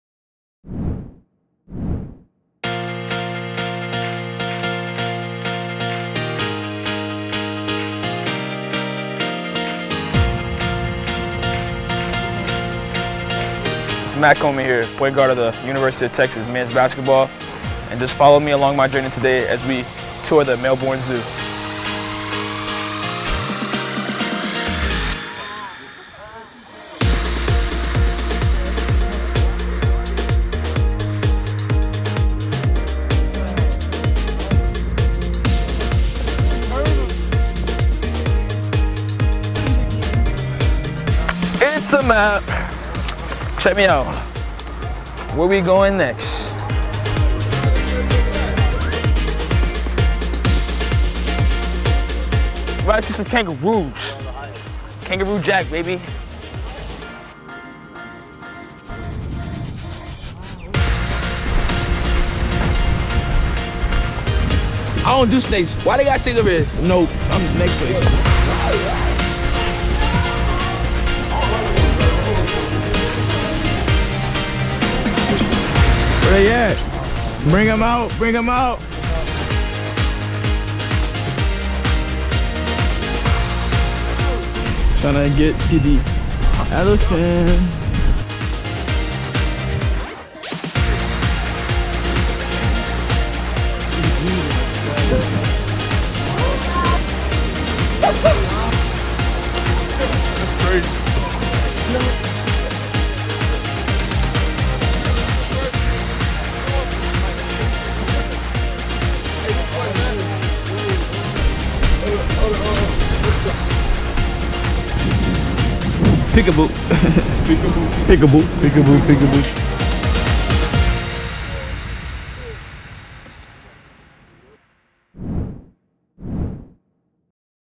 Texas Men’s Basketball Day 5 in Australia [August 16, 2017]
Freshman point guard Matt Coleman gives you a look at the team’s Wednesday morning tour of the Royal Melbourne Zoo.